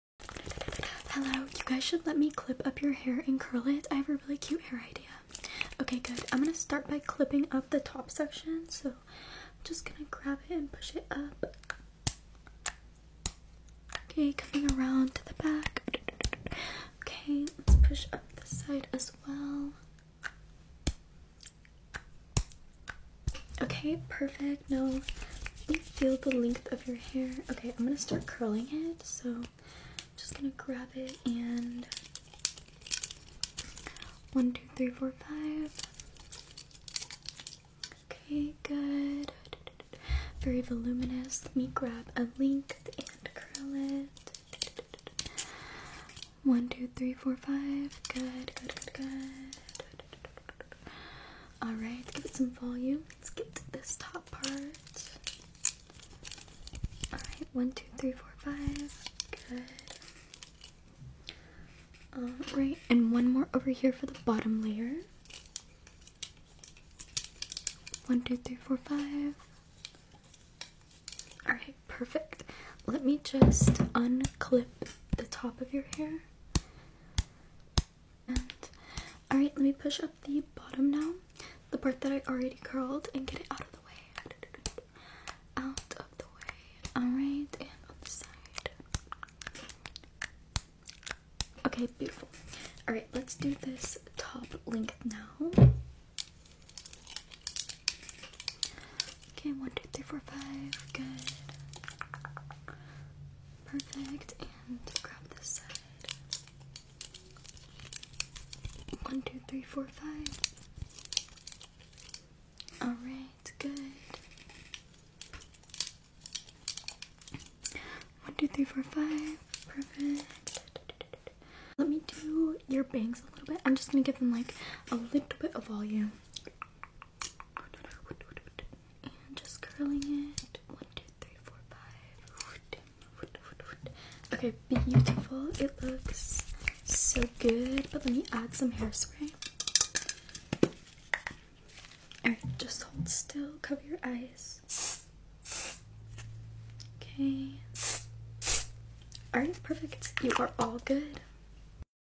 Another Hair video lol but lots of people loved the curling and clips!!asmr
Curling + clips = love 😍♥️